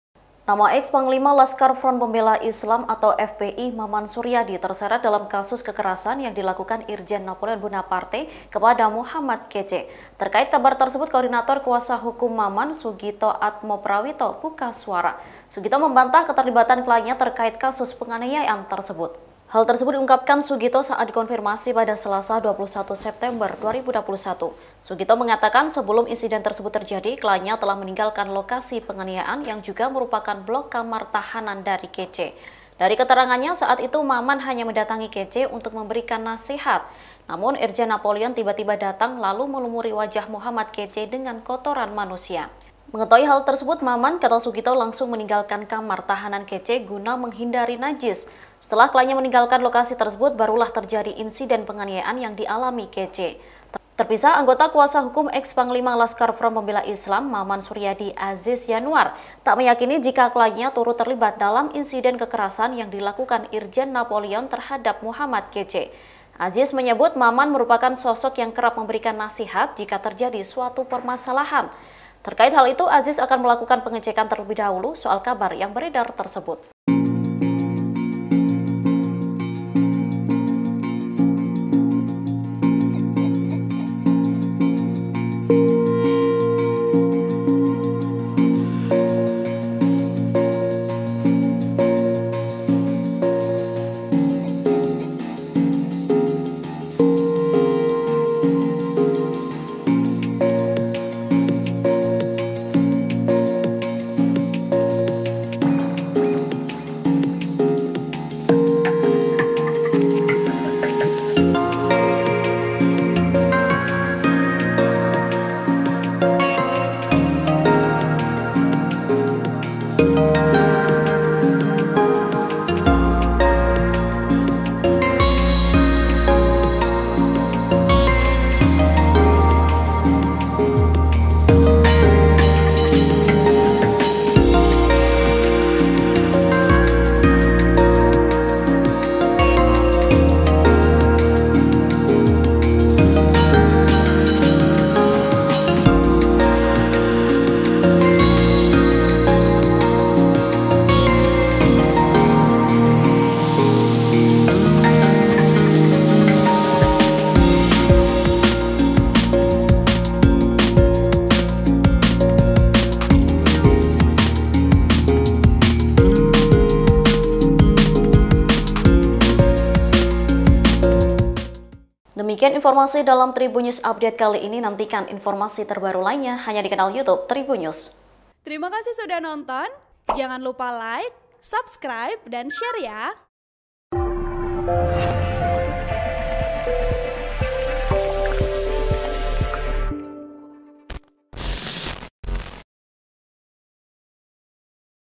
Eks Panglima Laskar FPI Dituding Terlibat Penganiayaan M Kece, Kuasa Hukum Berikan Bantahan
TRIBUN-VIDEO.COM - Nama eks Panglima Laskar Front Pembela Islam (FPI), Maman Suryadi terseret dalam kasus kekerasan yang dilakukan Irjen Napoleon Bonaparte kepada Muhammad Kece.

Terkait kabar tersebut, Koordinator kuasa hukum Maman, Sugito Atmo Prawito buka suara.

Sugito membantah keterlibatan kliennya terkait kasus penganiayaan tersebut.